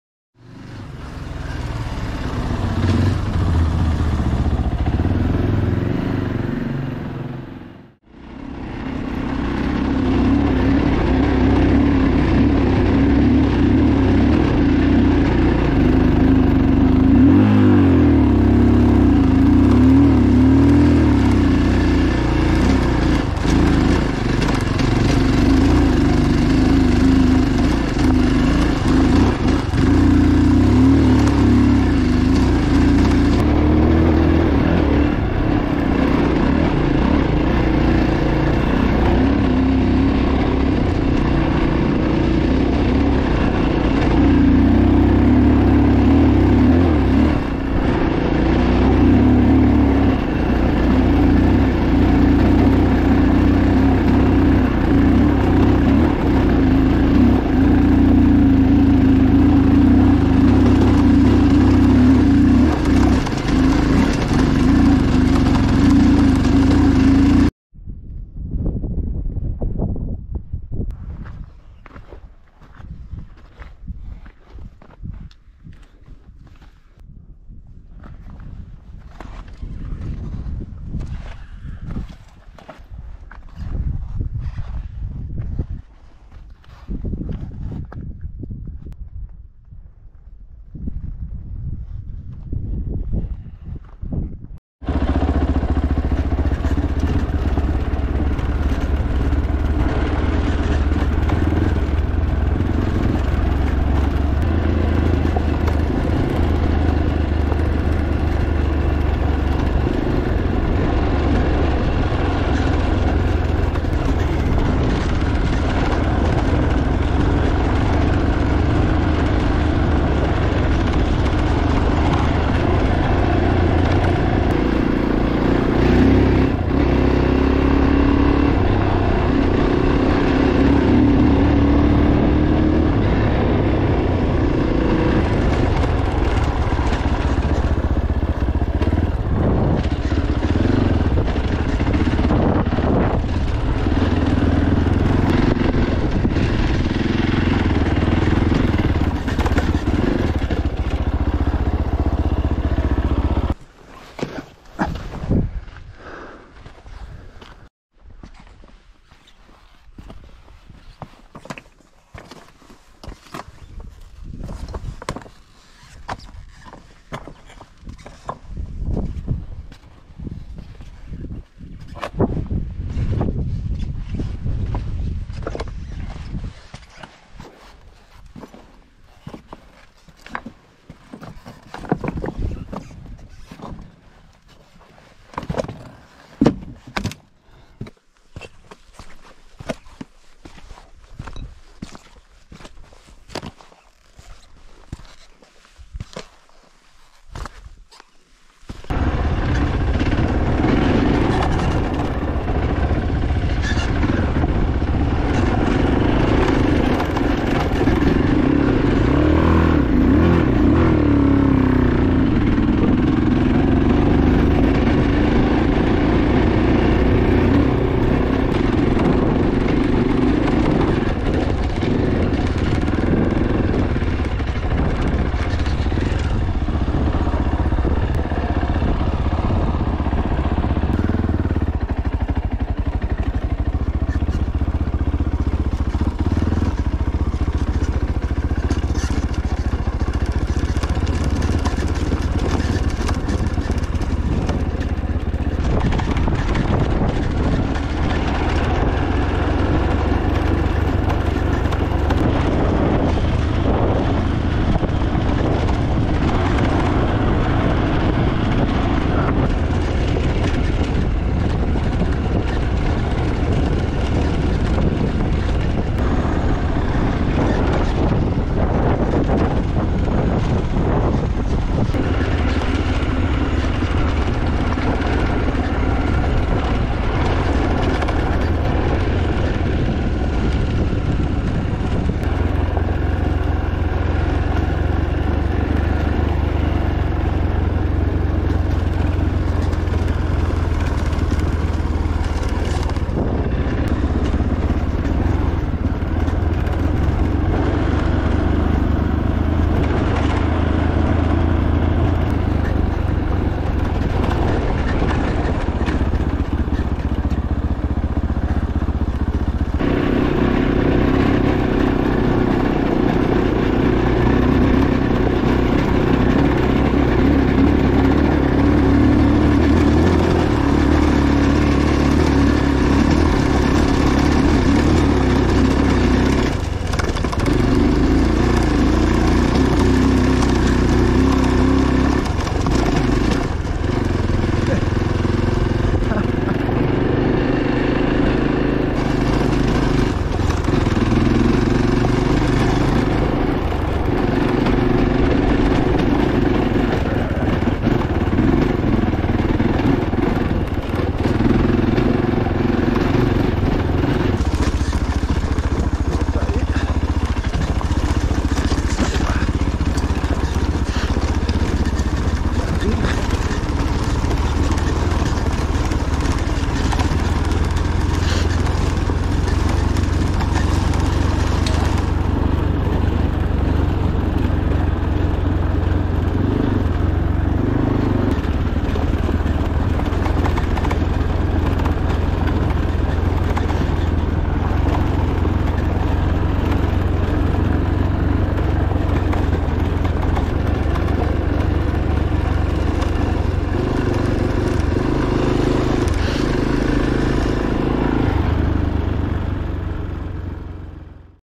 Transpirenaica Trail 2. Pic Negre VWT1 Andorra. Fantic Caballero 500 Rally 2K POV
Day 2: Setcases - Seu d'Urgell - Pic Negre - Seu d'Urgell (+ 200 km + extra)
2023-8-1

Video location: Pic Negre (Andorra)
00:00 a map
00:11 warming up
00:43 beautiful section
01:08 Fantic Sparrow Caballero 500 Rally. 7:30 pm. Pic Negre.
01:14 close up.
01:37 therapy
02:26 VW T1 Pic Negre
03:08 a pilgrim
03:19 messing around
03:39 antenna
04:06 letting the bike go
04:54 the sparrow met the swallow
05:10 last climbing section
05:40 bajada a Naturlandia

Notes:
(1) Follow an updated track. Entered Andorra by road (outdated track)
(2) to-do list: "la ruta de los contrabandistas"
(3) Toll in Naturlandia for bikes = - 25 €
(4) Roaming in Andorra = -€€